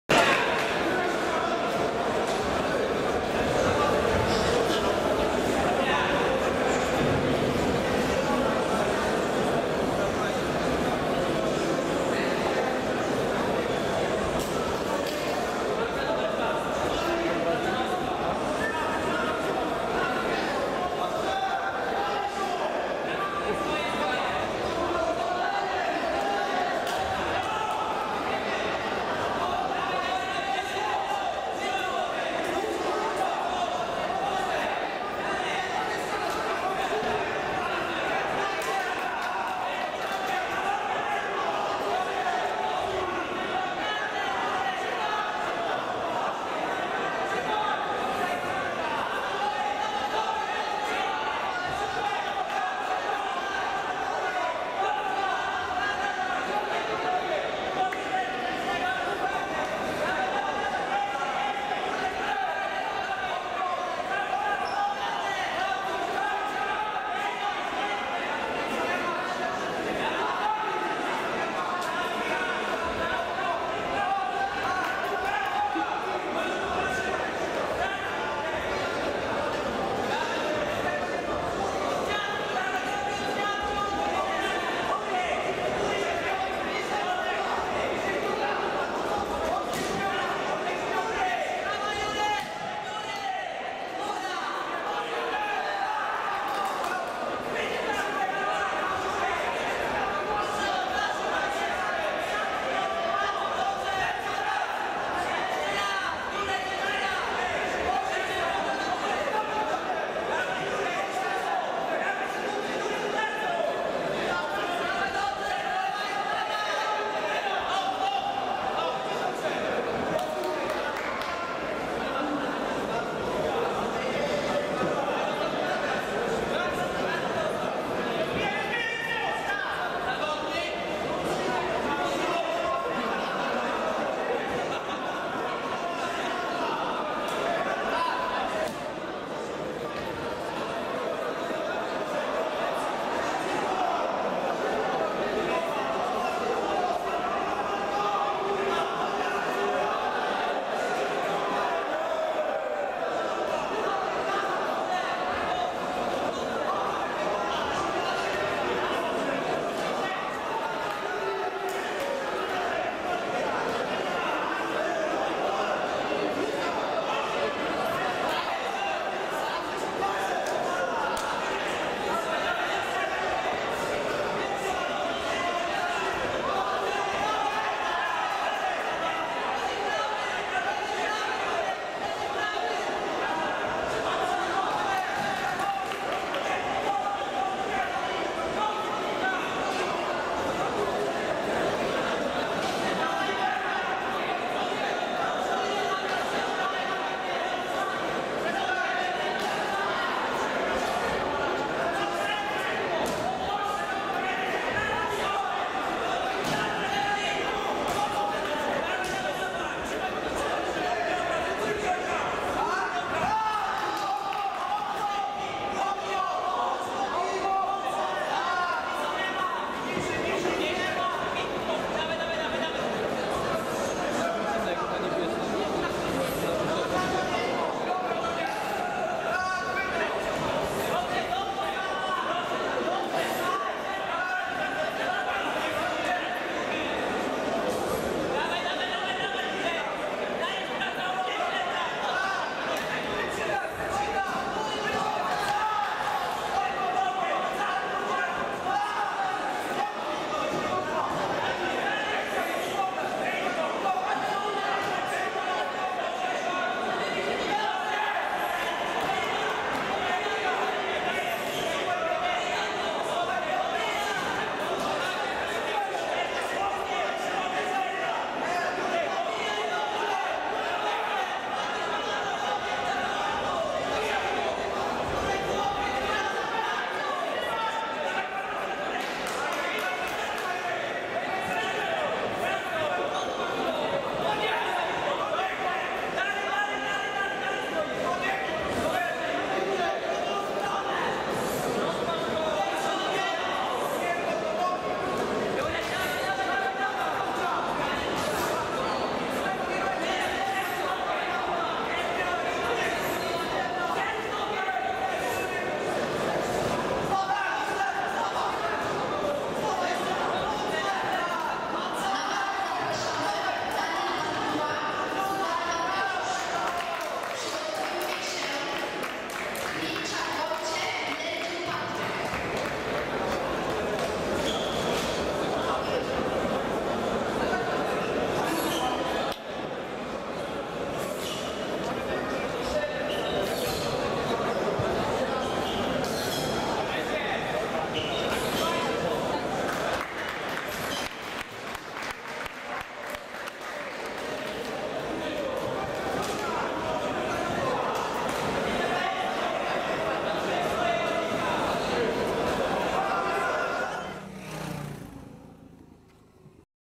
ALMMA 45: Jerzy Suski vs Grzegorz Tatarowski
- Portal o wszystkich sportach walki przedstawia pojedynek rozgrywany podczas gali Amatorskiej Ligi MMA 45 w Bochni - Jerzy Suski vs Grzegorz Tatarowski.